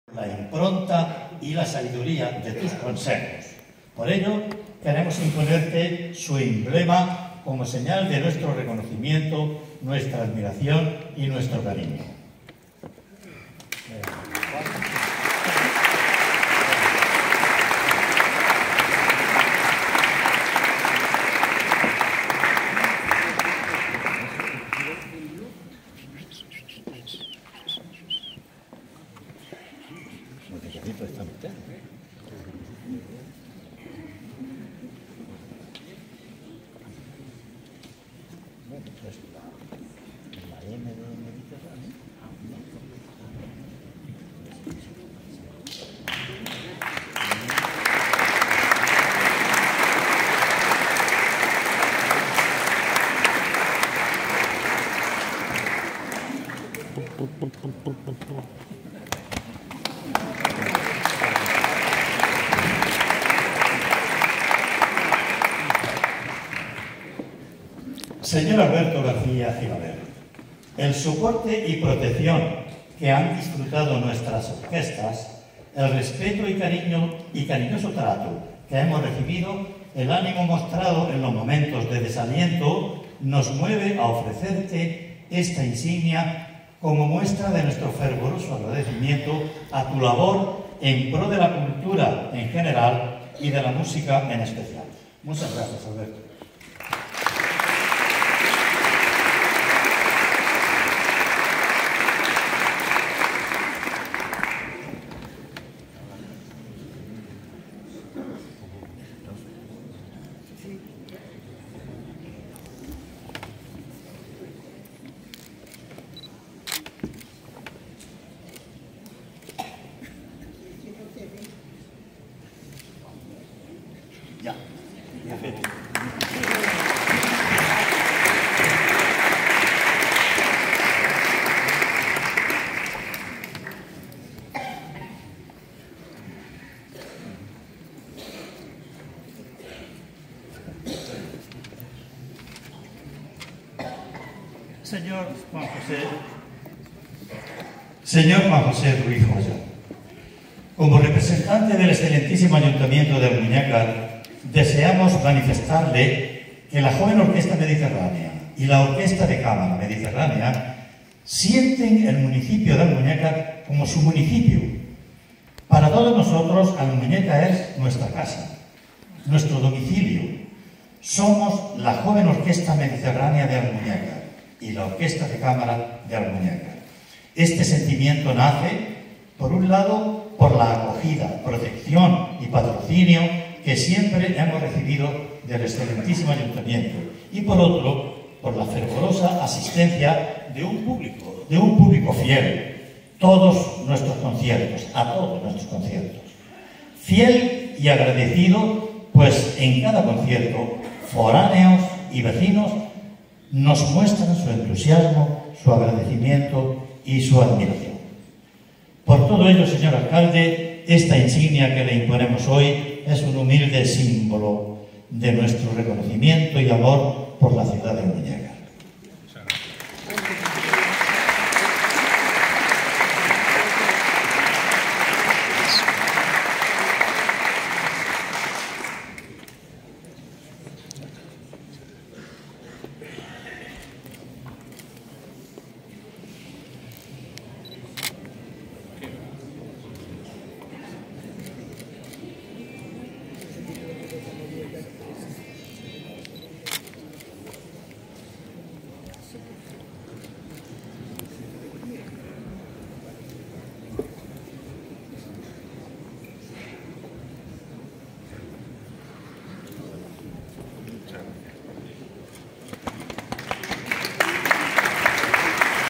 Joven Orquesta Mediterránea galardona a su Director, Alcalde y Concejal de Cultura de Almuñécar
Joven Orquesta Mediterránea galardona a su Director, Alcalde y Concejal de Cultura de Almuñécar en el Concierto de Año Nuevo en la Casa de la Cultura sexitana con el aforo casi completo.